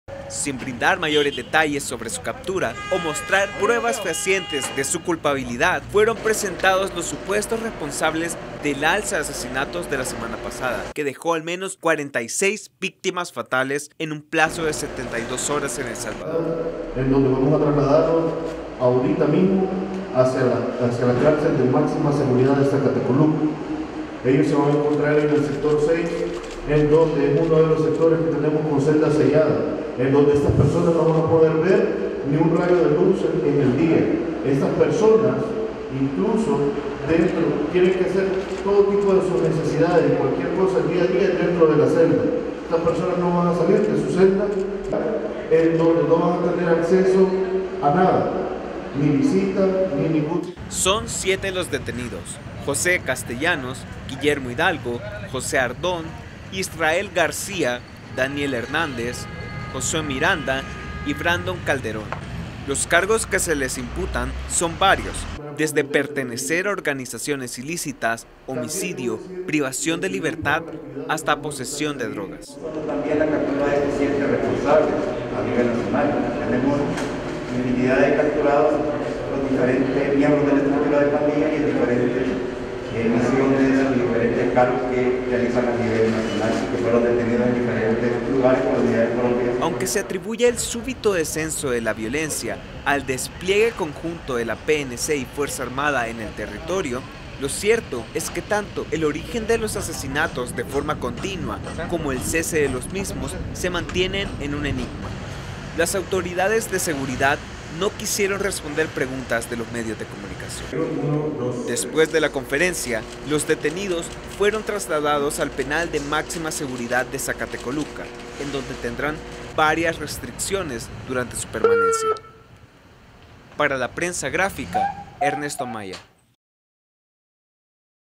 Presentan a supuestos responsables de la reciente alza de homicidios
Sin brindar mayores detalles sobre su captura o mostrar pruebas fehacientes de su culpabilidad, fueron presentados los supuestos responsables del alza de asesinatos de la semana pasada, que dejó al menos 46 víctimas fatales en un plazo de 72 horas en El Salvador.

¡Visítenos!